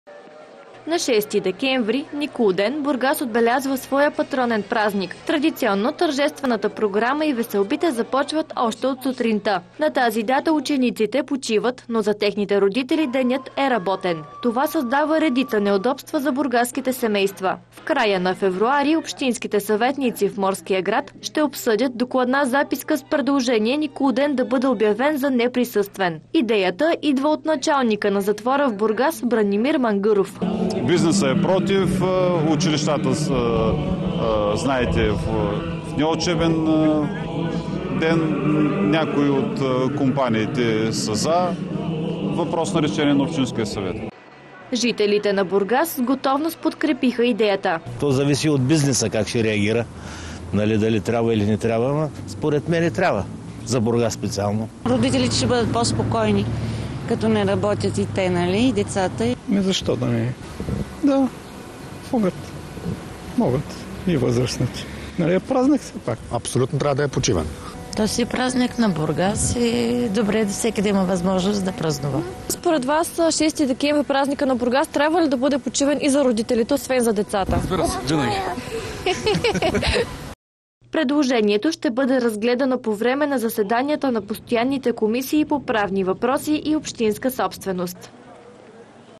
Никулден – неработен ден за бургазлии?
Да бъде ли празникът на Бургас – Никулден, неработен за жителите на морския град? Това ще решават общинските съветници на редовна сесия в края на февруари. Какво мислят бургазлии по темата, вижте в репортажа